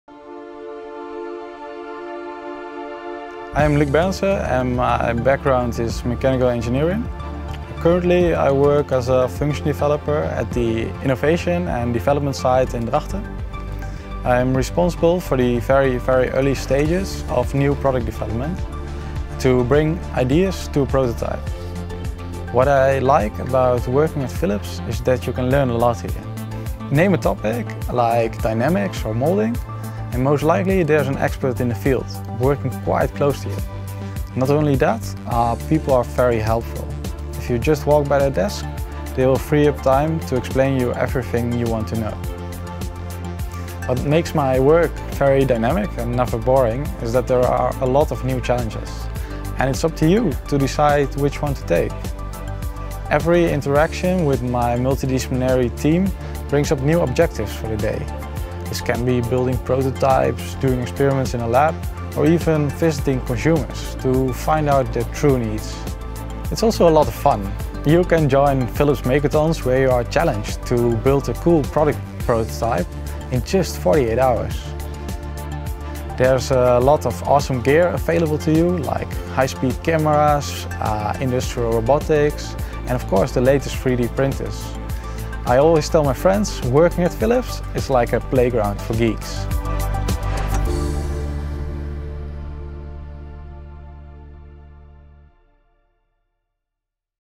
Challenge yourself at Philips: Mechanical engineering careers
At Philips it's not just about building a product, it's about building a healthier society. As Luc says: "I always tell my friends, working at Philips is like a playground for geeks."